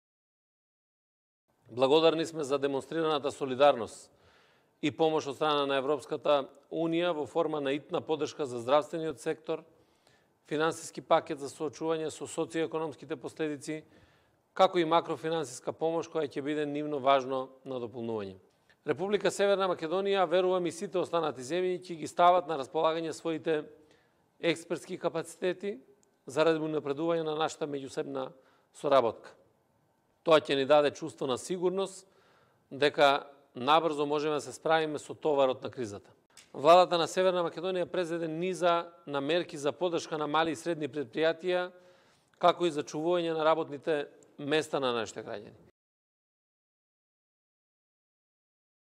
Спасовски на самитот на ЦЕИ
Претседателот на Владата на Република Македонија Оливер Спасовски денеска учествуваше на вонредниот самит на Централно европската иницијатива посветена на договорите за соработка против пандемијата од КОВИД-19.